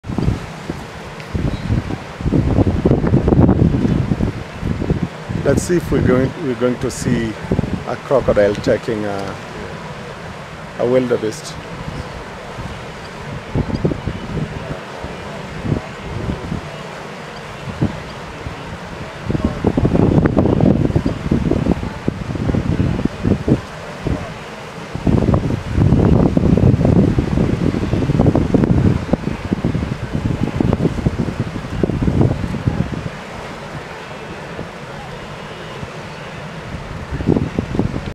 The Wildebeest Migration River Crossing Tanzania
One of the best places to observe the wildebeest migration is at the Mara River. The Mara River runs through Kenya and Tanzania and lies directly in the path of migration for multiple animals in the Masai Mara & Serengeti National Park.